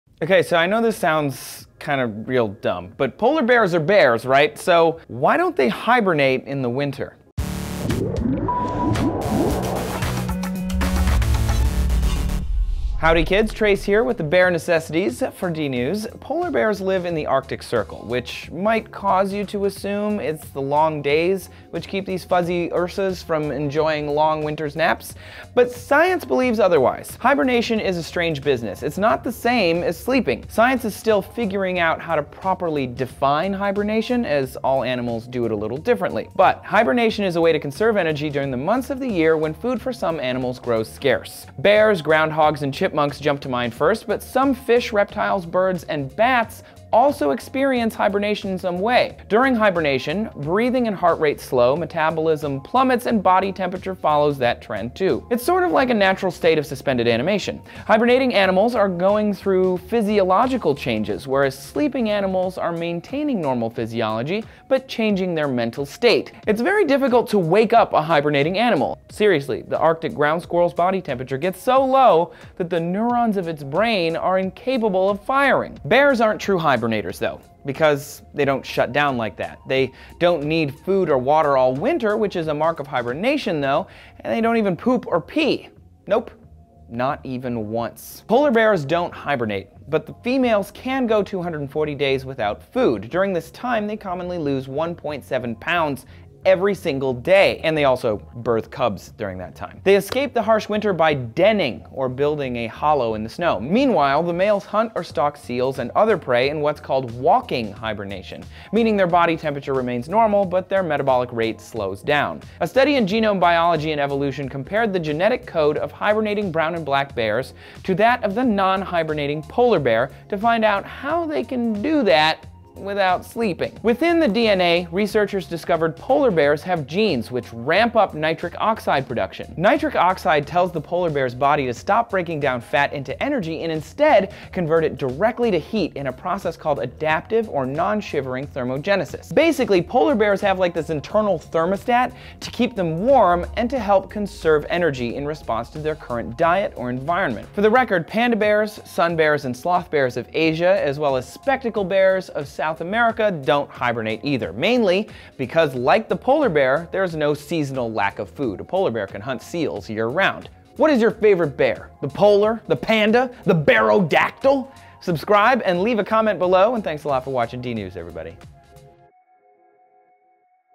Why Polar Bears Don't Hibernate
Many different types of species hibernate, and they all do it a little bit differently. Many bears hibernate, but for some reason, polar bears do not. Why is this? Trace explains why polar bears don't need to hibernate, and also explains how hibernation works in a few different species.

Why Polar Bears Don't Need To Hibernate
"Brown and black bears hibernate during winter to conserve energy and stay warm. But the same isn't true for polar bears."

Polar Bears Exhibit Genome-Wide Signatures of Bioenergetic Adaptation to Life in the Arctic Environment
"Polar bears (Ursus maritimus) face extremely cold temperatures and periods of fasting, which might result in more severe energetic challenges than those experienced by their sister species, the brown bear (U. arctos)."

"Every living animal on Earth is burning energy all the time."

"Hibernation. Polar bears do not overwinter in dens like brown and black bears."

"Bears hibernate. They spend all year eating salmon, blueberries, and picnic baskets and then, sometime around baseball playoffs, they all wander to a cave full of treasure and explorers' skulls where they curl up in a big furry ball and snore away the winter."

Polar and brown bear genomes reveal ancient admixture and demographic footprints of past climate change
"Polar bears (PBs) are superbly adapted to the extreme Arctic environment and have become emblematic of the threat to biodiversity from global climate change."